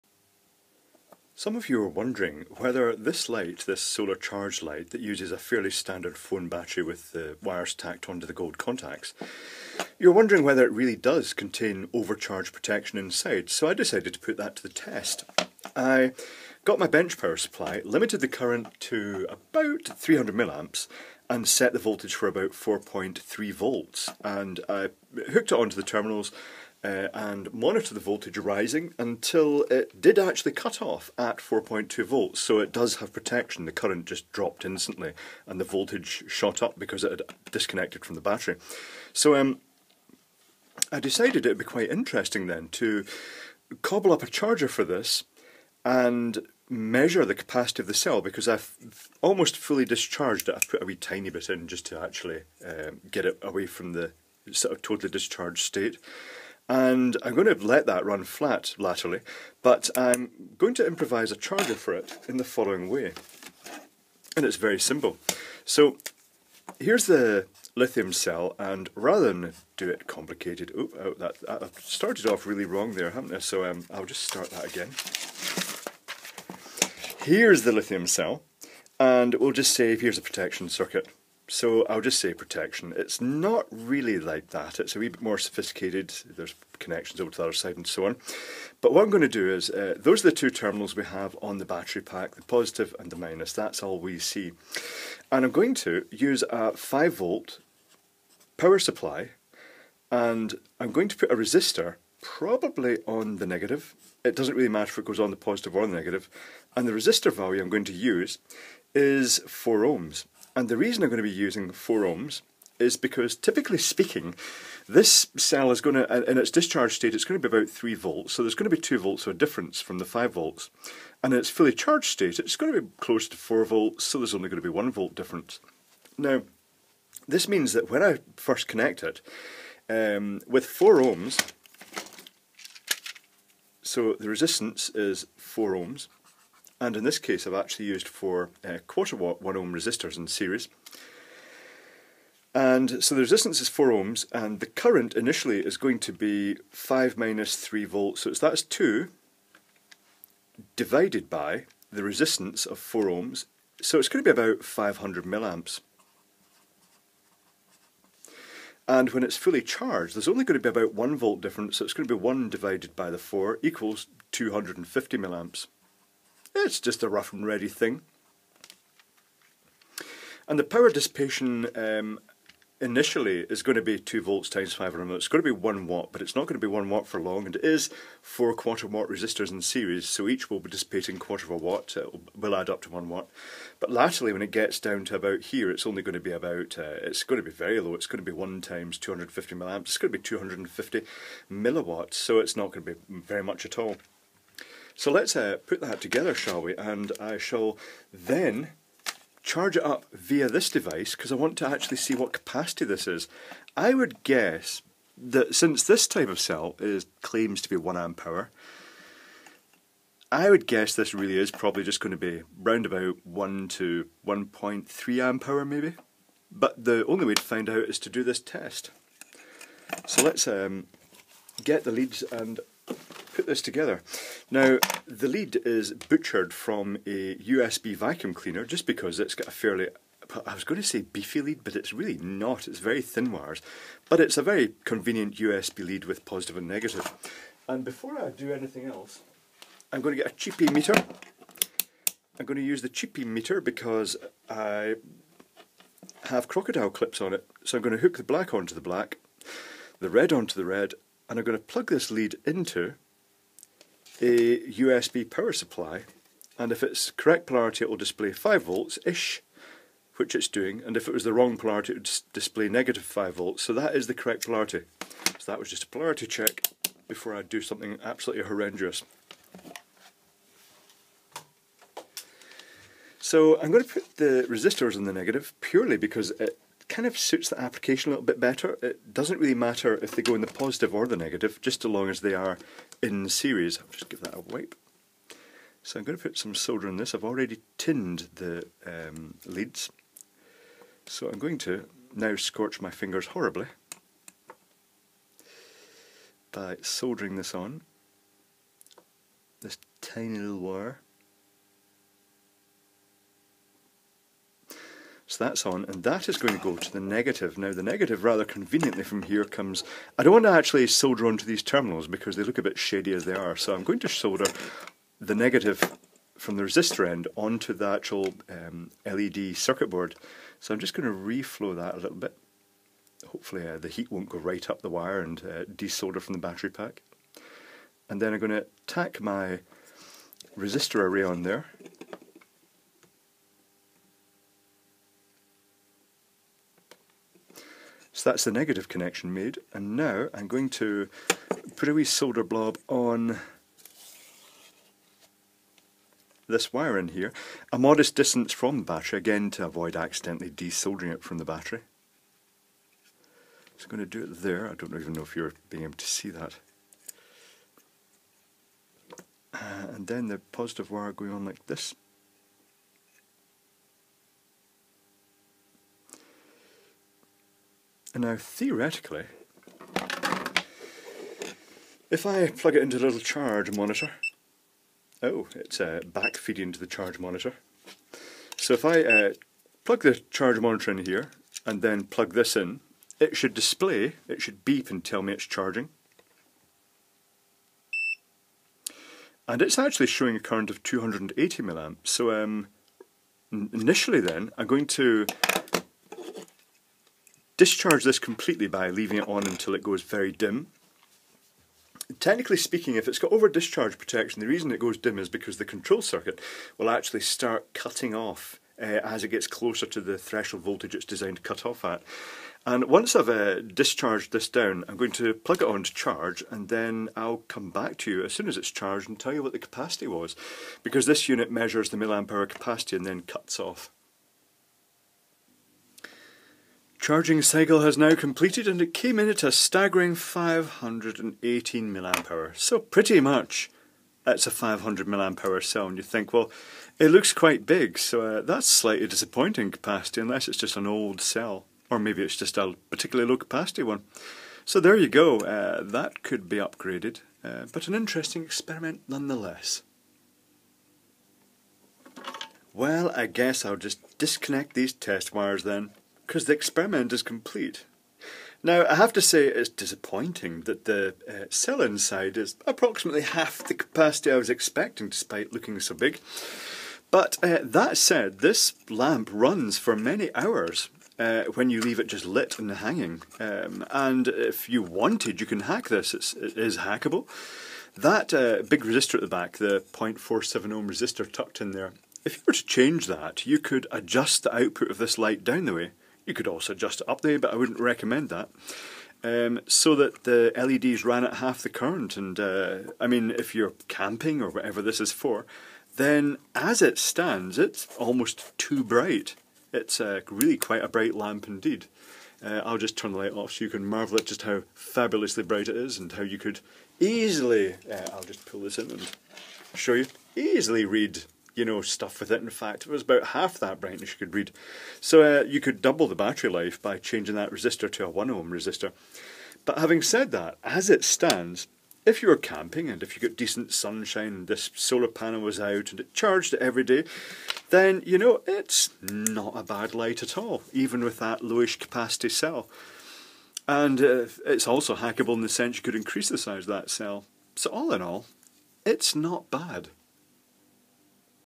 Lithium cell capacity test on a solar charged lamp.
This started off as a test to see if the re-purposed lithium phone cell in a solar charged LED lamp did have built-in overcharge protection.
It then evolved into a lithium cell capacity test with an improvised USB charge hack.
By request, here's a link to the ebay listing for this lamp.